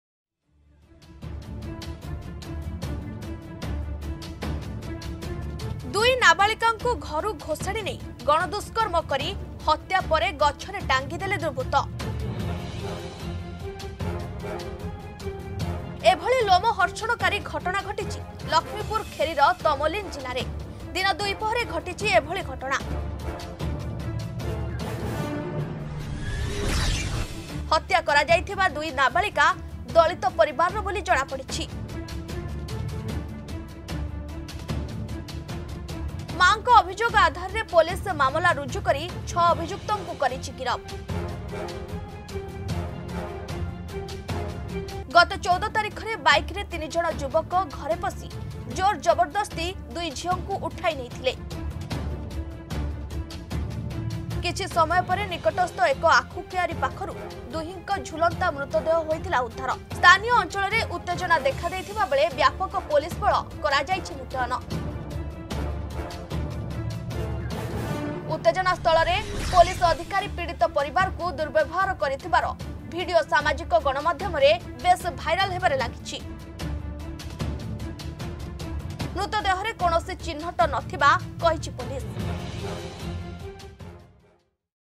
ଘରୁ ଘୋଷାଡି ନେଇ ଗଣଦୁଷ୍କର୍ମ ପରେ ଗଛରେ ଟାଙ୍ଗି ଦେଲେ | Kalinga Scroll
ସ୍ଥାନୀୟ ଅଞ୍ଚଳରେ ଉତ୍ତେଜନା ଦେଖାଦେଇଥିବା ବେଳେ ବ୍ୟାପକ ପୋଲିସ ବଳ କରାଯାଇଛି ମୁତୟନ । ଉତେଜନାସ୍ଥଳରେ ପୋଲିସ ଅଧିକାରୀ ପୀଡିତ ପରିବାକୁ ଦୁର୍ବ୍ୟବହାର କରିଥିବାର ଭିଡିଓ ସାମାଜିକ ଗଣମାଧ୍ୟମରେ ବେଶ ଭାଇରାଲ ହେବାରେ ଲାଗିଛି । ମୃତଦେହରେ କୌଣସି ଚିହ୍ନଟ ନଥିବା କହିଛି ପୋଲିସ ।